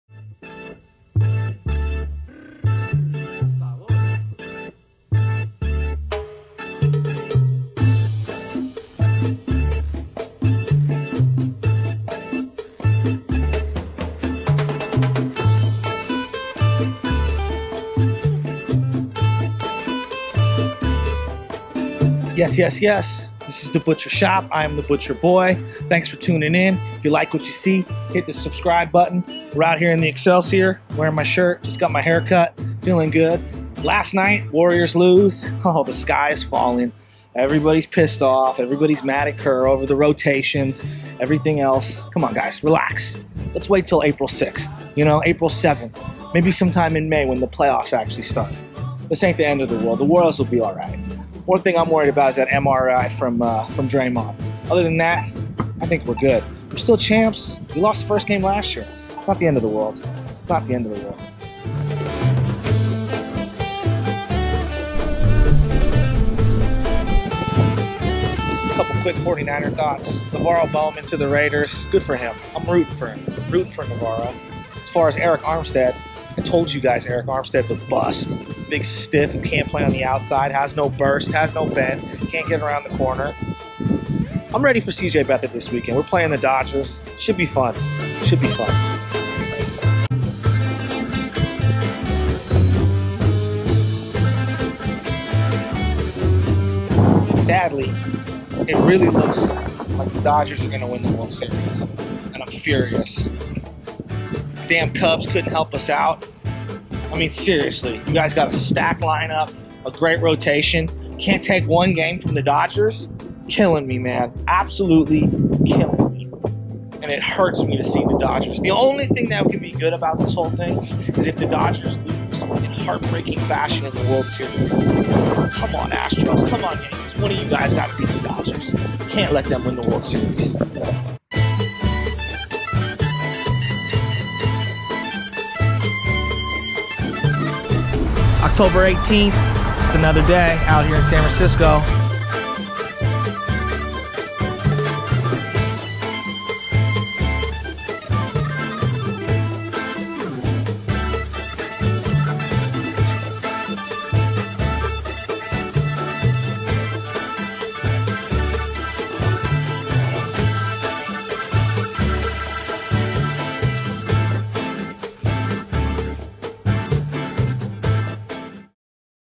Excelsior District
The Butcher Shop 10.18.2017
Warriors lose, Draymond tweaks knee, 49ers tid bits